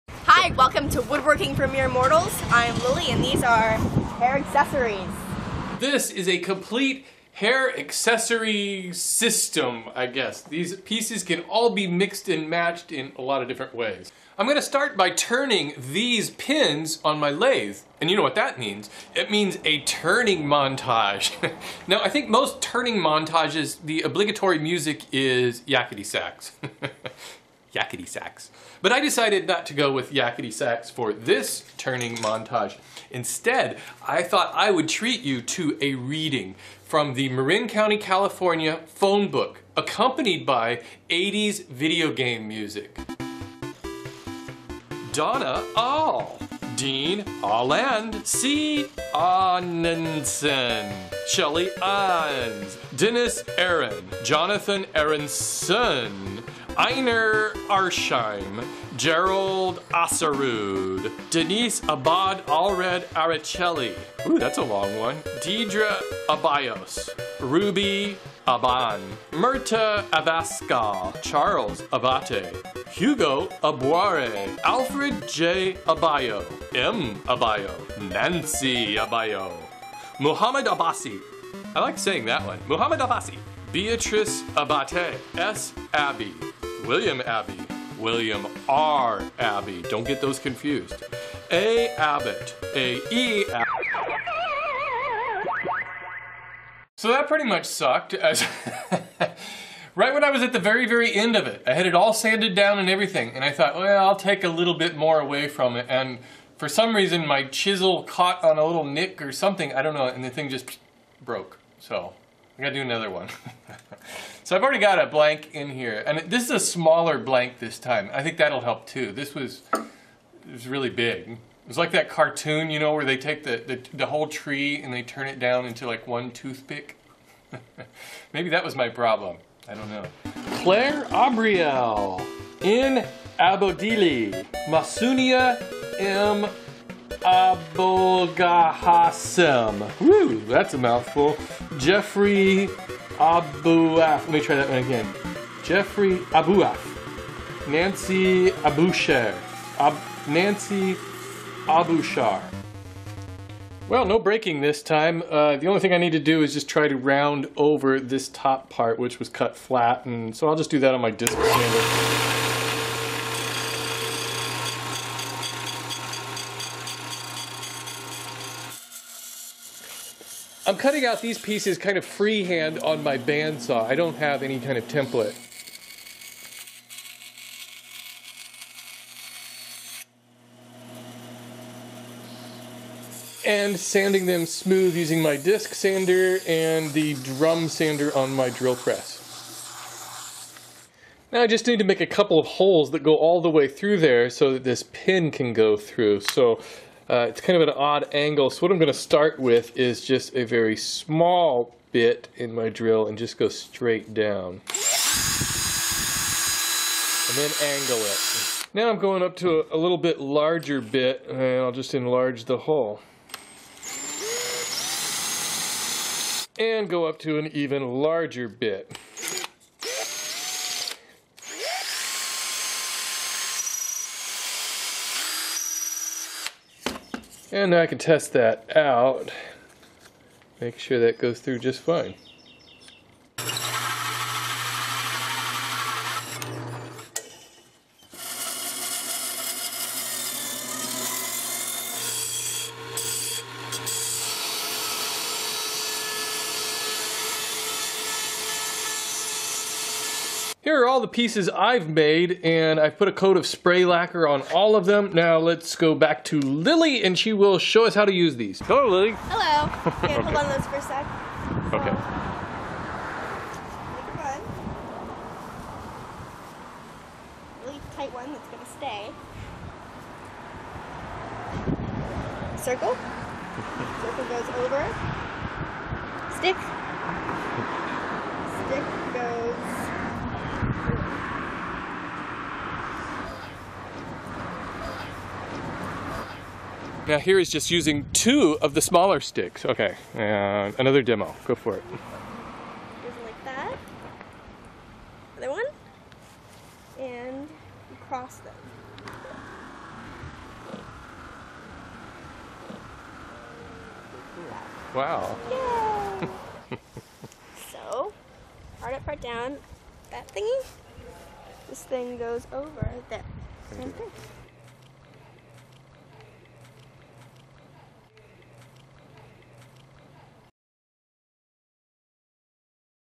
Wood hair accessories
Here are several hair clips and rings you can mix and match. Easy to make and a great gift idea. If you don't have a lathe, you could just use some dowels.

MAILING ADDRESS:

WWMM
Category
Howto & Style